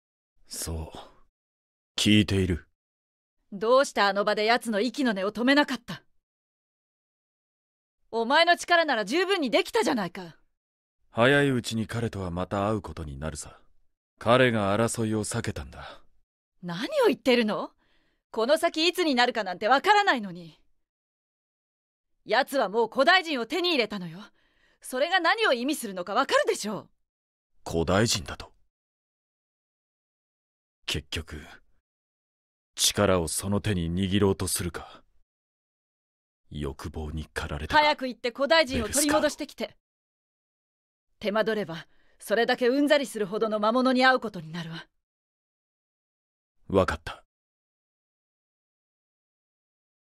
Argenta and Geraint meeting (Japanese)
The subtitle it's off, should say "yes, I can hear you".

Anyways, Japanese dialogue seems different so subtitles here aren't really gonna be helpful.

Argenta: Atsuko Tanaka
Geraint: Toshiyuki Morikawa